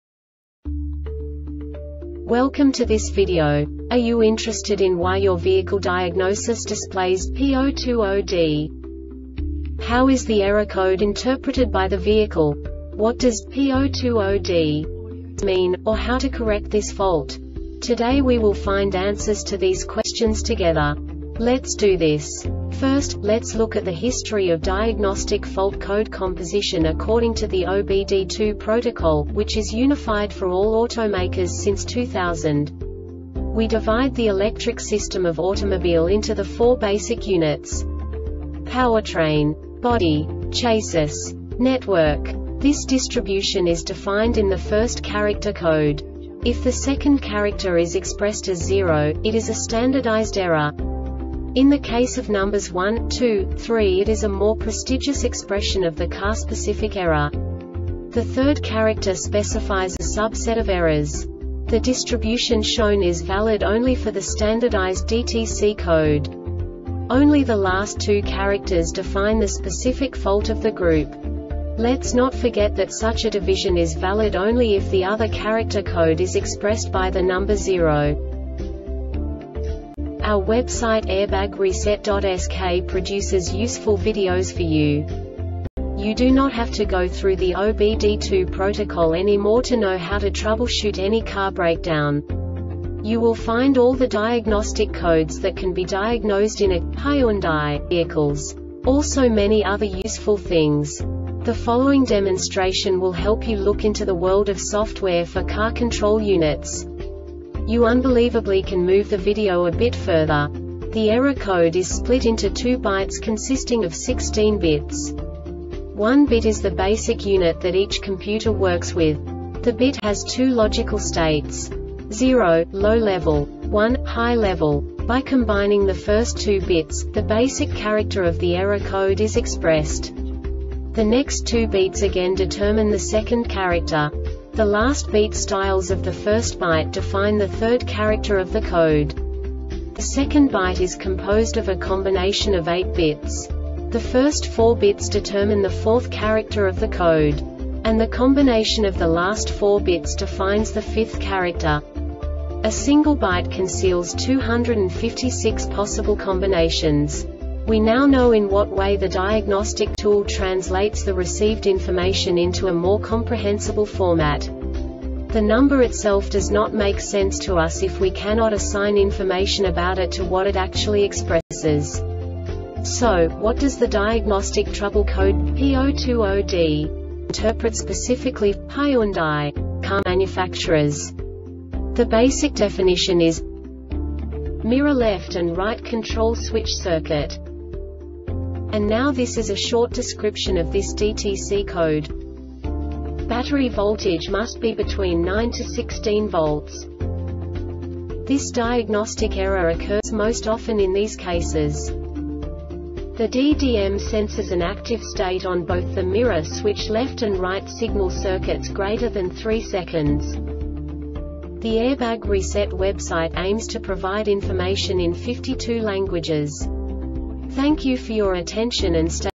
DTC Hyundai P020D Short Explanation
Contents:
0:21 Basic DTC analysis according to OBD2 protocol standard.
1:48 Insight into programming
2:58 A diagnostically understandable description of the Diagnostic Trouble Code
3:05 Basic error definition

Make:
Hyundai
Code:
P020D
Definition:
Mirror Left and Right Control Switch Circuit
Description:
Battery voltage must be between 9-16 volts.
Cause:
The DDM senses an active state on both the mirror switch left and right signal circuits greater than 3 seconds.